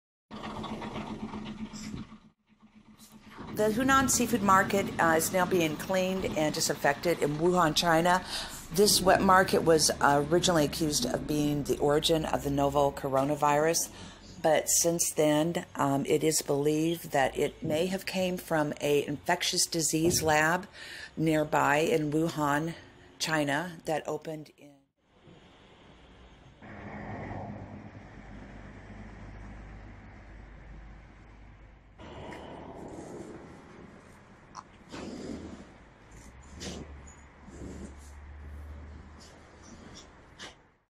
The Huanan Market Being Prepared Reopen Wuhan China Coronavirus Pandemic outbreak
The Huanan Market is being cleaned and sprayed with disenfectant, as they prepare for it's reopen in Wuhan China. It was originally believed that this wet market was the source of the beginning of the novel Coronavirus. However, since then it has come to light that this conversation virus may have indeed been made in a lab. In 2017 a lab was opened in Wuhan China, to study dangerous infectious diseases. It was open even though there was many who opposed it's opening in Wuhan and who tried to prevent it from opening. They pointed out that dangerous infectious diseases had escaped such labs before with deadly results. Of course, they opened the lab anyways. It was suspected that this lab was studying and creating bio-warfare.

Many countries have such labs, including the United States.

God bless 🙏